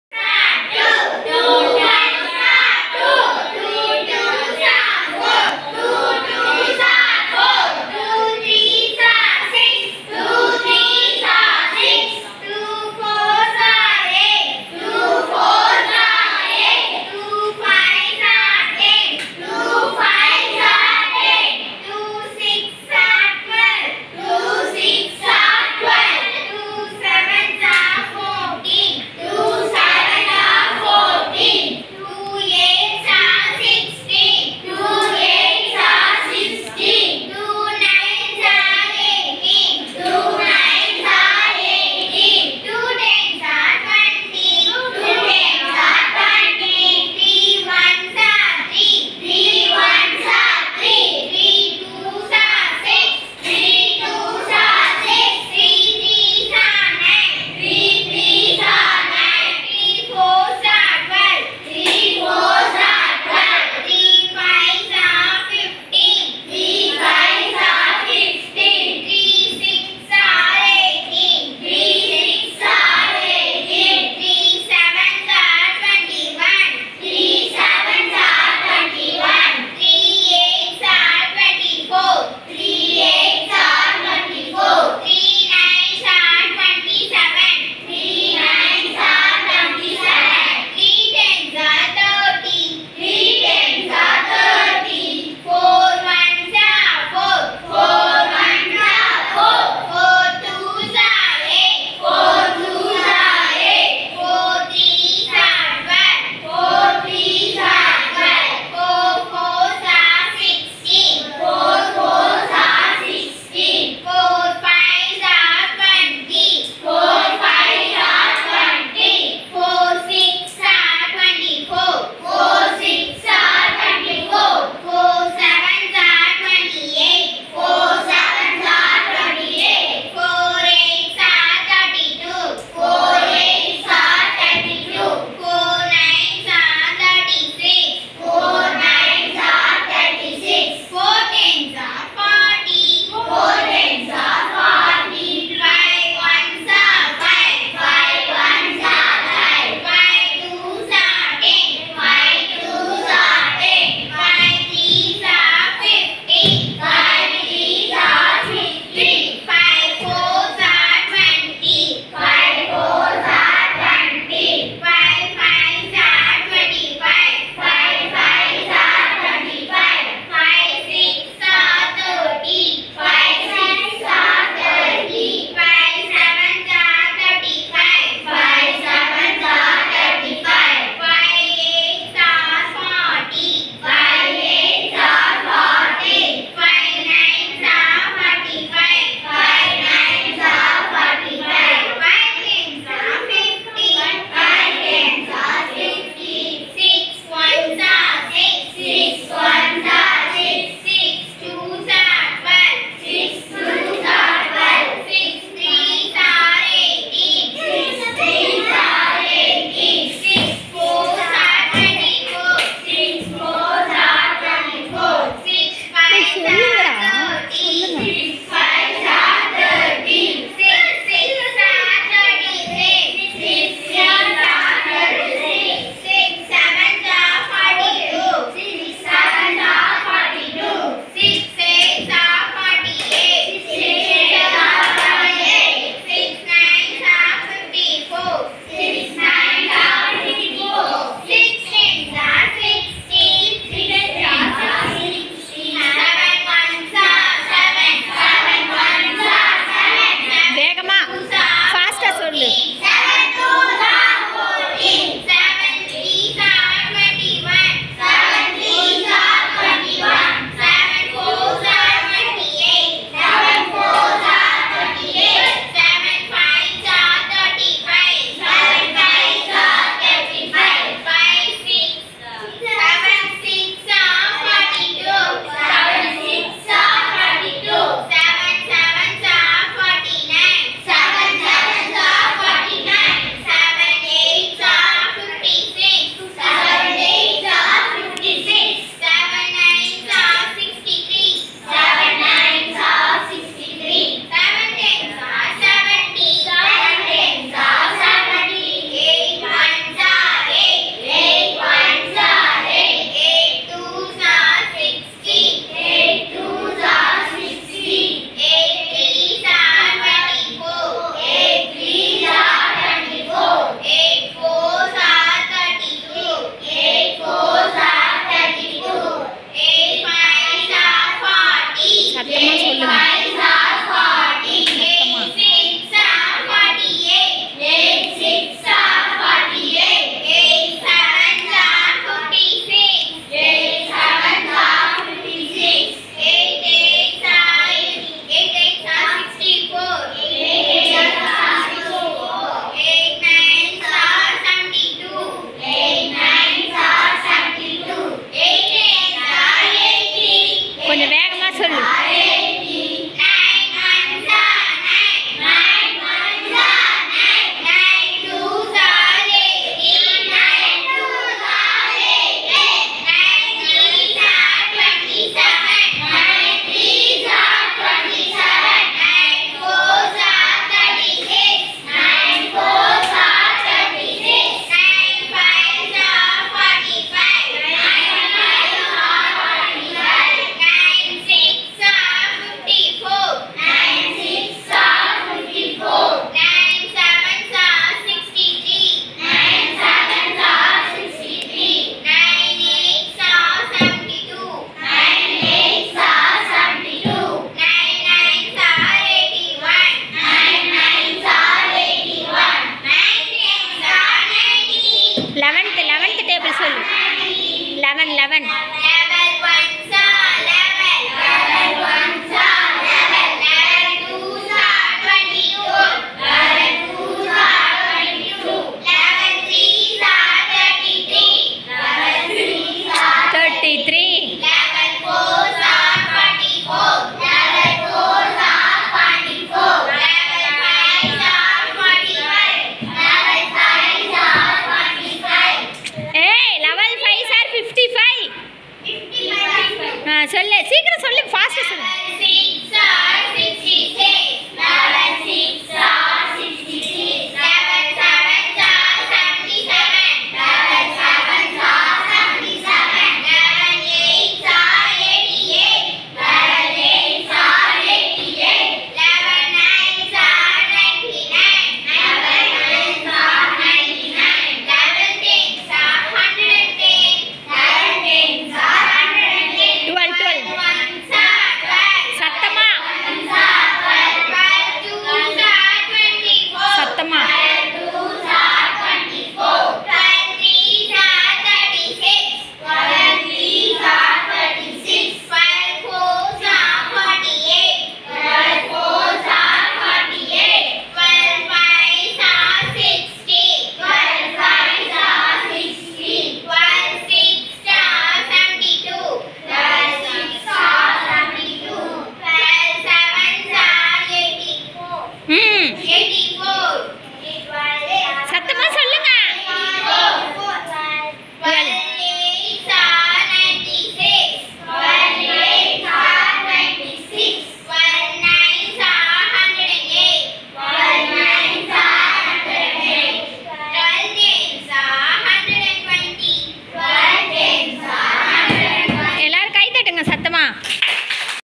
இல்லம் தேடி கல்வி
இல்லம் தேடி கல்வி வகுப்பு
வாய்ப்பாடு ஒப்புவித்தல்
ஐந்தாம் வகுப்பு மாணவ மணிகள்